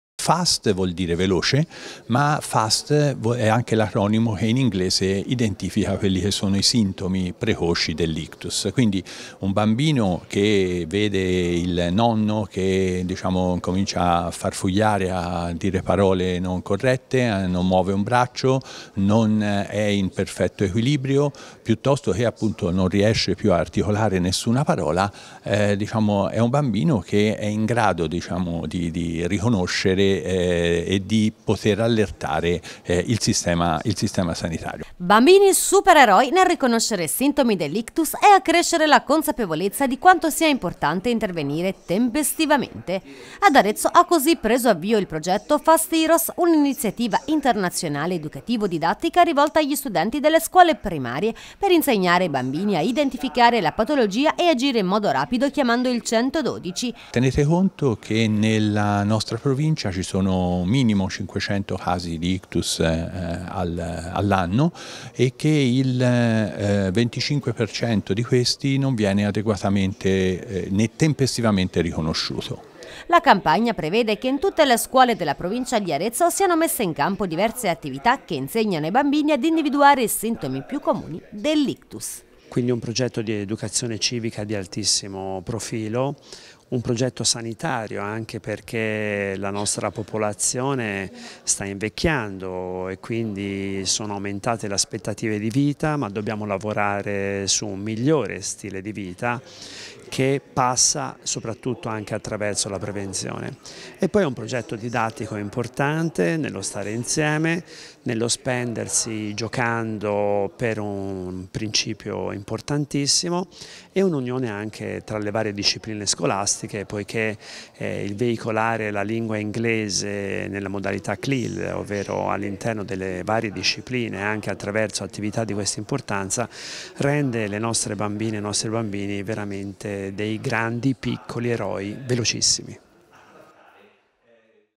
Bambini “supereroi” nel riconoscere i sintomi dell’ictus: al via il progetto Fast Heroes
Riconoscere i sintomi dell’ictus e accrescere la consapevolezza di quanto sia importante intervenire tempestivamente. È questo l’obiettivo di Fast Heroes, un’iniziativa internazionale educativo-didattica, portata avanti in Italia da A.L.I.Ce. Italia Odv (Associazione per la Lotta all’Ictus Cerebrale), rivolta agli studenti delle scuole primarie per insegnare ai bambini a identificare il “trombo malefico” e agire in modo rapido chiamando il 112. Il progetto - promosso dai Dipartimenti Emergenza Urgenza e Educazione alla Salute della Asl Toscana sud est insieme all'Ufficio Scolastico Provinciale coinvolgerà gli Istituti Comprensivi della provincia di Arezzo.